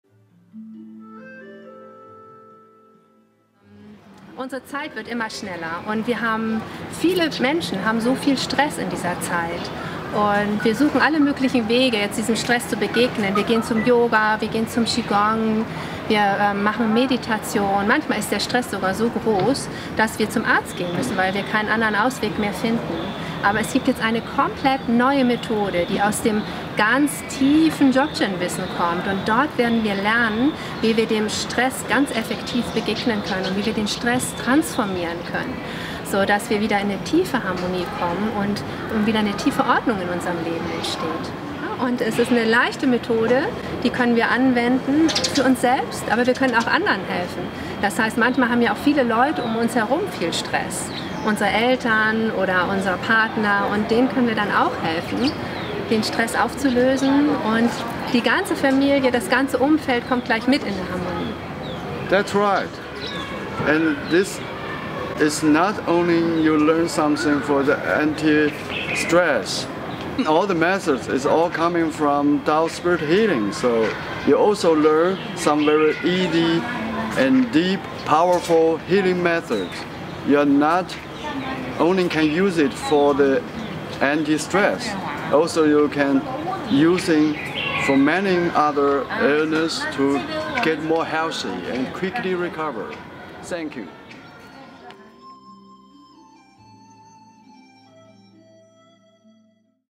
Anti-Stress-Coaching mit Dao Spirit-Healing - Fr., 5.7. – So., 7.7. 2019
Anti-Stress-Coaching mit Dao Spirit-Healing:
Unser Leben verändert sich immer schneller und stellt immer höhere Anforderung an uns. Stress scheint fast unvermeidlich. In dieser Zeit benötigen wir eine schnelle und effektive Methode. Meister Shangshi hat auf den Wurzeln der uralten buddhistischen Dzogchen-Tradition ein unglaublich wirksames Anti-Stress-Programm entwickelt, mit dem wir uns selbst und andere behandeln können. Dem Stress wird nicht nur wirksam begegnet, sondern die negativen Kräfte des Stresses werden transformiert und stehen uns als Potential wieder zur Verfügung. Mit einem starken und gesunden Körper, einem klaren Geist und einem friedvollen Herz können wir einfach den Alltag bewältigen und haben eine gute und stabile Grundlage für eine spirituelle Entwicklung.

In diesem Seminar wird Meister Shangshi die tiefen Ursachen von Stress erklären und unsere Sichtweise über Stress vollkommen verändern. Er wird uns in eine einzigartige Methode der Heilung einführen, die es nie zuvor gab: Dem Dao-Spirit Healing und das daraus entwickelte Anti-Stress-Programm, das sehr wirksam Erschöpfung, Burnout, Schlafprobleme und viele andere stressbedingte Störungen auflösen kann. Die Essenz dieser Heilmethode lag lange verborgen in der Dzogchen-Tradition des tibetischen Buddhismus. Meister Shangshi ist der erste, der diese Methode öffentlich macht.

Mit der Kraft seines erwachten Herzens wird Meister Shangshi ein starkes Feld aufbauen, in dem er uns das wahre Sein erfahren lässt. Er wird uns den Schlüssel zu unserer eigenen Heilfähigkeit überreichen, der in jedem von uns schlummert, wir nur oft keinen Zugang dazu haben. Er wird unsere Fähigkeit wecken, uns selbst und andere Menschen zu heilen – ob körperlich, psychisch und auch emotional.

Diese Form von Behandlung ist sehr flexibel und kreativ einsetzbar. Die Anwendung ist anstrengungslos, spielerisch und trotzdem sehr wirksam. Wer behandelt, verliert keine Energie, sondern wird mitbehandelt. Anfänger bekommen eine fundierte Einführung und können nach dem Seminar sofort behandeln. Fortgeschrittene vertiefen ihre Fähigkeiten und bekommen durch das Feld und die Herz-Unterstützung von Meister Shangshi und seiner kompletten Dzogchen-Linie die Chance auf eine große innere Veränderung.

Ziel des 3-Tages-Seminars: Alle Teilnehmer bekommen eine Übertragung der 4-Sekunden-Enlightenment-Methode und eine fundierte Einführung in das Antistress-Programm. Ihre Fähigkeit zur Lichtmassage und ein Lichtfeld aufzubauen wird geöffnet, und sie können diese Methoden anwenden. Meister Shangshi wird mit uns viele praktische Übungen durchführen, uns persönlich anleiten und korrigieren. In Partnerarbeit werden wir die Heilmethode vertiefen.
Der Kurs wird von Chinesisch auf Deutsch übersetzt, mit gelegentlichen Ergänzungen auf Englisch.

Kurszeiten: Fr., 5.7. – So., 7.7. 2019, je 10.00 – 18.00 Uhr (inkl. Mittagspause)
Kursort: Mozart-Säle, Moorweidenstr. 36, 20146 Hamburg
Infos: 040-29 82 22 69 (Dr. Weihua Dohnke) / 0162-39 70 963 (Andrea Thiele).